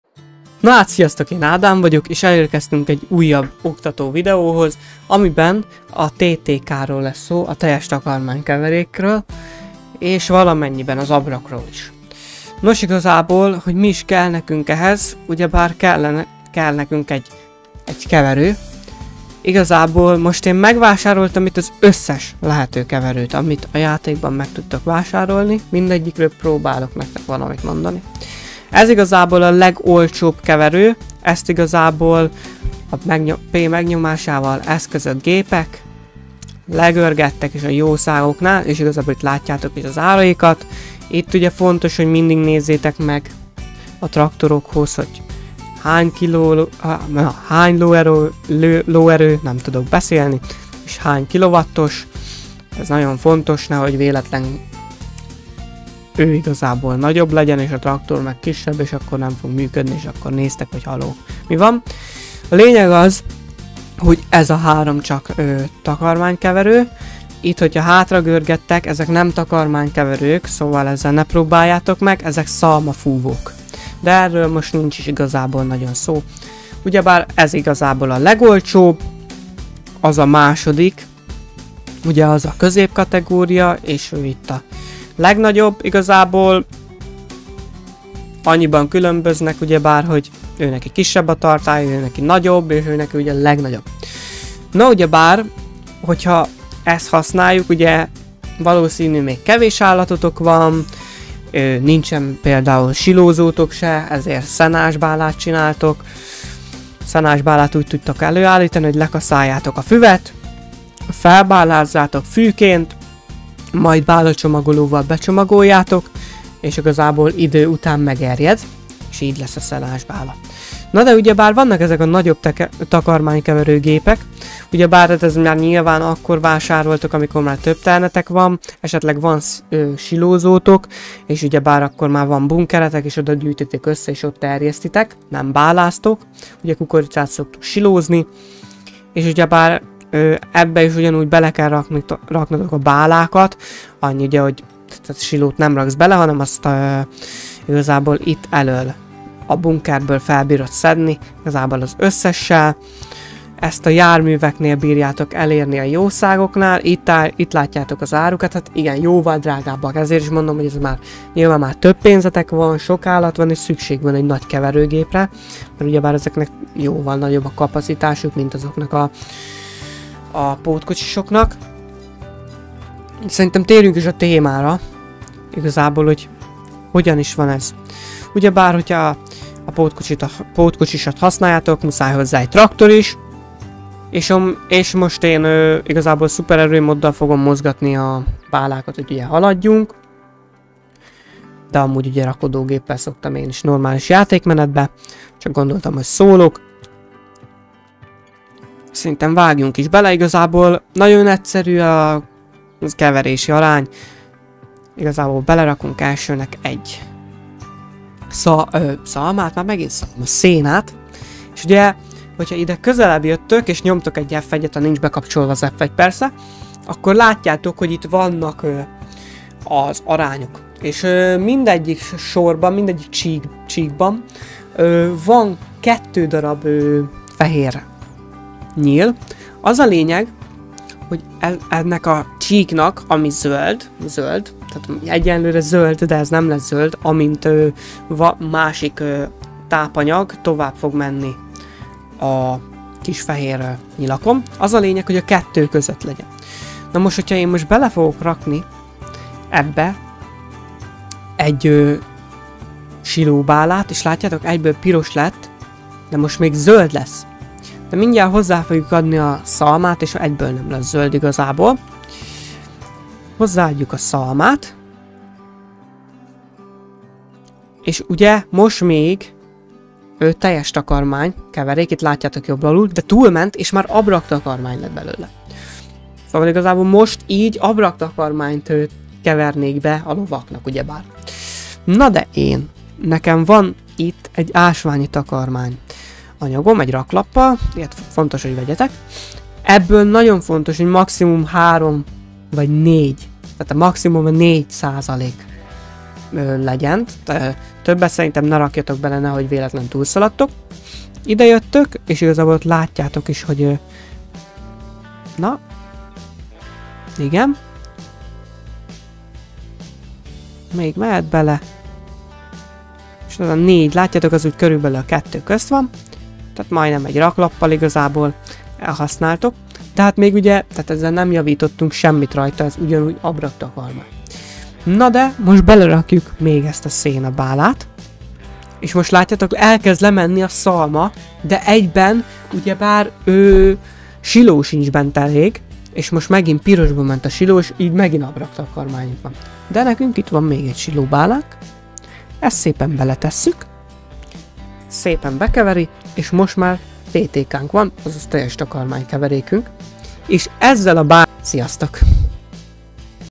Teljes takarmánykeverék keverési arányok [TUTORIAL]
Sziasztok a teljes takarmánykeverési arányokról lesz szó a videóban remélem tudtam segíteni jószórakozást a videóhoz sziasztok!